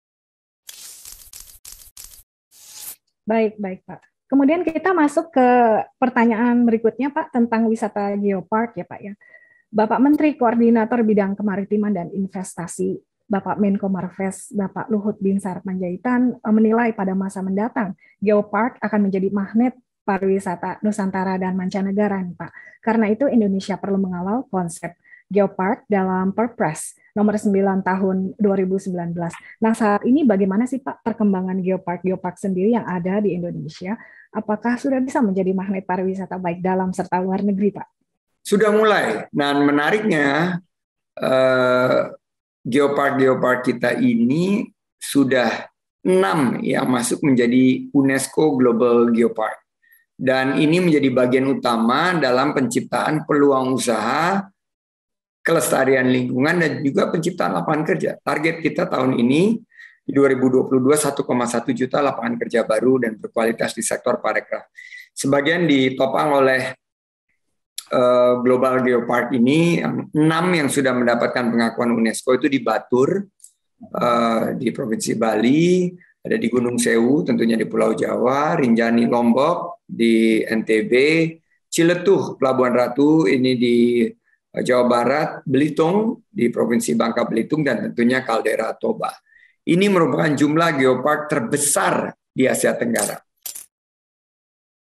6 Geopark Indonesia yang Diakui UNESCO jadi Peluang Usaha hingga Penciptaan Lapangan Kerja
Menteri Pariwisata dan Ekonomi Kreatif (Menparekraf), Sandiaga Salahuddin Uno menyebutkan ada 6 destinasi wisata di Indonesia yang ditetapkan sebagai UNESCO Global Geoparks. Pertama, Geopark Batur yang berada di Pulau Bali. Kedua, Geopark Pegunungan Sewu yang terletak di Pulau Jawa. Ketiga, Geopark Gunung Rinjadi di NTB. Keempat, Geopark Ciletuh di Jawa Barat. Kelima, Geopark Belitong di Belitung dan keenam, Geopark Danau Toba di Sumatera Utara.

Sandi mengatakan ditetapkannya 6 destinasi wisata tersebut sebagai UNESCO Global Geoparks menjadi bagian utama dalam penciptaan peluang usaha, kelestarian lingkungan dan penciptaan lapangan kerja. Menparekraf menargetkan di tahun 2022 ada 1,1 juta lapangan pekerjaan baru dan berkualitas di sektor pariwisata dan ekonomi kreatif.